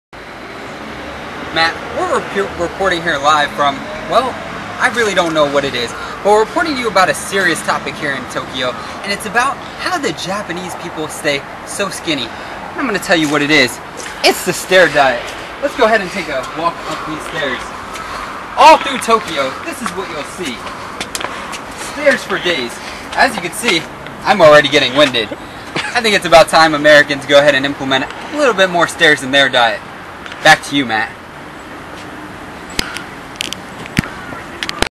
Funny Tokyo Diet Channel 13 Tim Bream (2012)
This is a funny video that discusses the excessive amount of stairs found all throughout Tokyo Japan. We found a connection between the size of the average Japanese person and the amount of stairs. I traveled with a friend of mine and toured the entire city having fun taking videos throughout out journey. This shows the fun side of Ninja Tim.